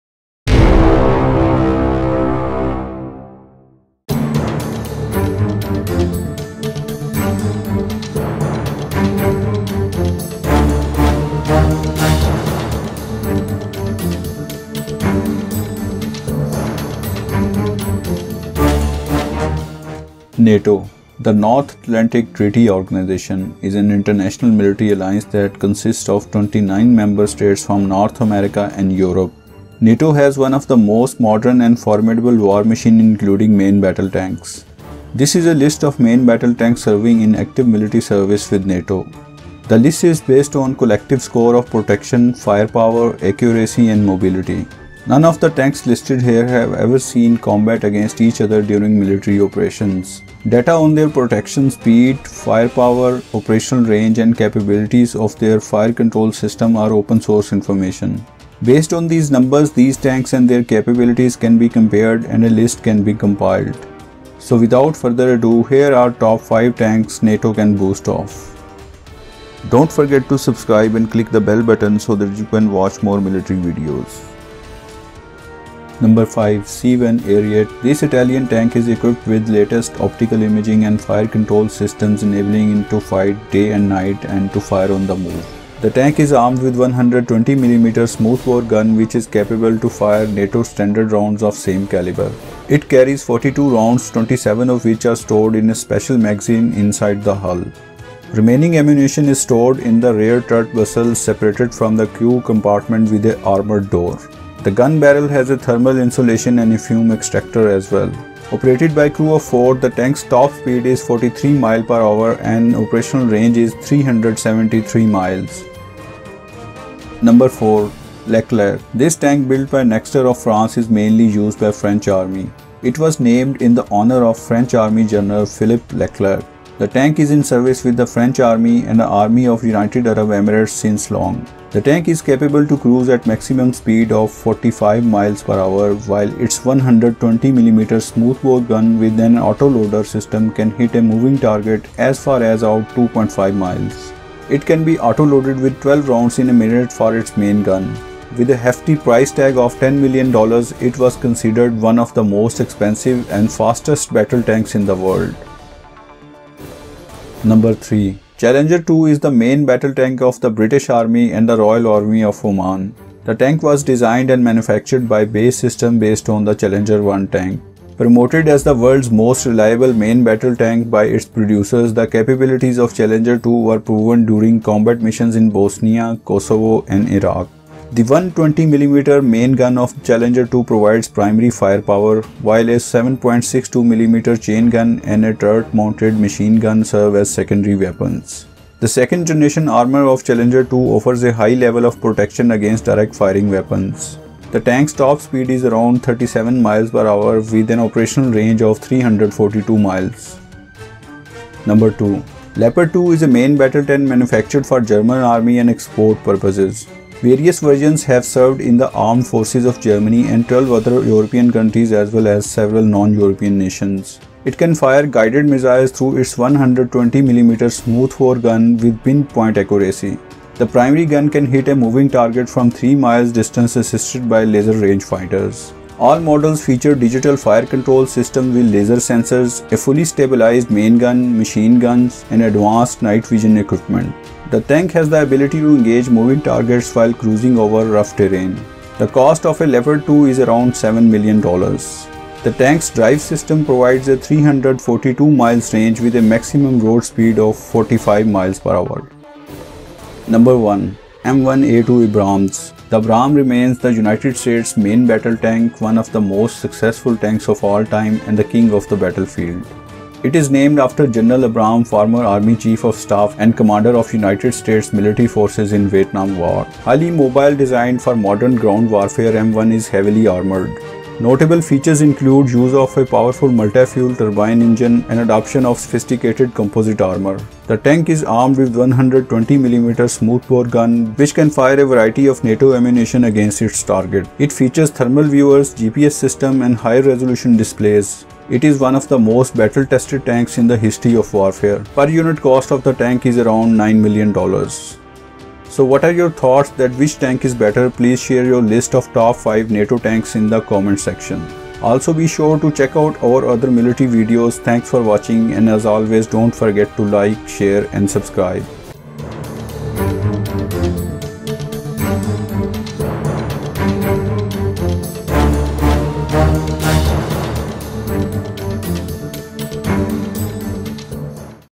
Top 5 Main Battle Tanks of NATO
NATO (The North Atlantic Treaty Organization) is an international military alliance that consists of 29 member states from North America and Europe. NATO has one of the most modern and formidable war machine including main battle tanks. This is a list of main battle tanks serving in active military service with NATO...

5. C1 Ariete, Italy
This Italian Tank is equipped with the latest optical imaging and fire control systems enabling it to fight day & night and to fire on the move. The tank is armed with 120 mm smoothbore…

4. Leclerc, France
This tank built by Nexter of France is mainly used by french army. It was named in honour of french army General Philippe Leclerc. The tank is in service with the French Army and the army of United Arab Emirates …

3. Challenger 2, GB
Challenger 2 is the main battle tank of the British Army and the Royal Army of Oman. The tank was designed and manufactured by BAE Systems based on the Challenger 1 tank. Promoted as the world's…

2. Leopard 2, Germany
The Leopard 2 is a main battle tank manufactured for German Army and export purposes. Various versions have served in the armed forces of Germany and 12 other European countries as well as several non-European nations…

1. M1A2 Abrams, USA
The Abrams remains the US's main battle tank, one of the most successful tanks of all time, and the king of the battlefield. It is named after General Abrams former Army chief of staff, and commander of United States military forces in the Vietnam War…

So what are your thoughts that which tank is better? Please share your list of Top 5 NATO Tanks in the comments section.

Credits

► Esercito Italiano
► Recrutement armée de Terre
► Bundeswehr
► British Army
► US Department of Defense & US Army
► NATO

The appearance of visual information does not imply or constitute the respective country or its army’s endorsements.